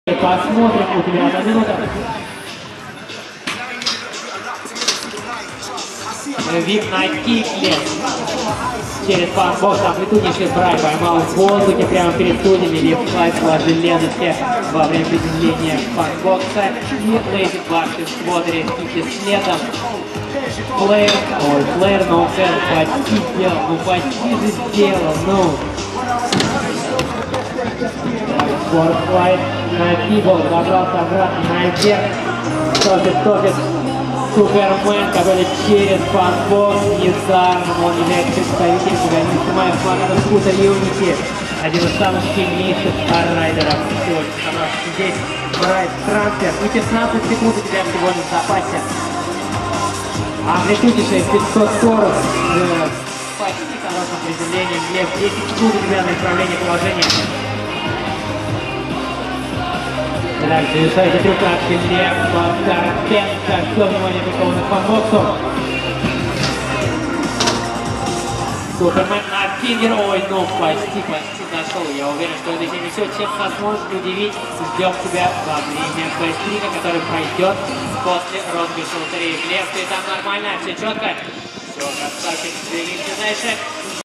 Карпенко Глеб - финал 2п. Чемпионат России по самокату 20170506
6 мая 2017 г.Москва в скейтпарке ПКиО "Кузьминки" прошел Чемпионат России по самокату - Russian Scooter Championship 2017.